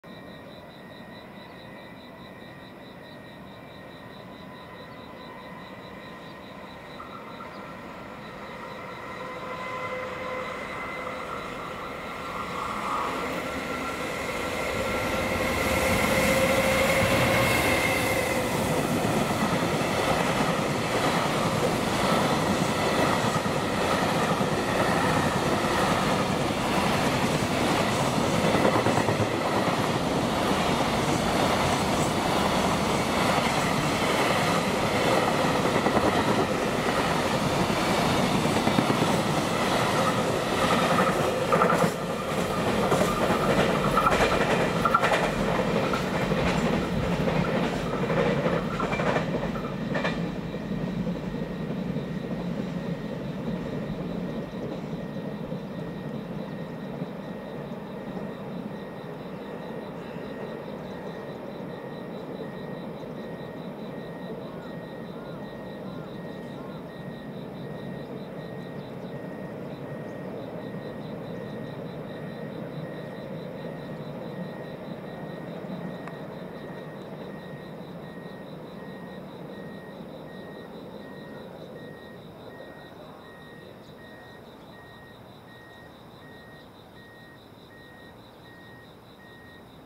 吹桃 西濃とJOT白コンテナ多数積載【4K】山陽本線 上り 貨物列車 3050レ EF210-314号機 [吹] 牽引 コンテナ JR貨物 吹田機関区 西河原就実 2022.9.4 M5760001
吹桃 西濃とJOT白コンテナ多数積載 山陽本線 上り 貨物列車 3050レ EF210-314号機 [吹] 牽引 コンテナ JR貨物 吹田機関区 西河原就実 2022.9.4 M5760001

3050レ 幡生～大阪 吹田機関区 A194仕業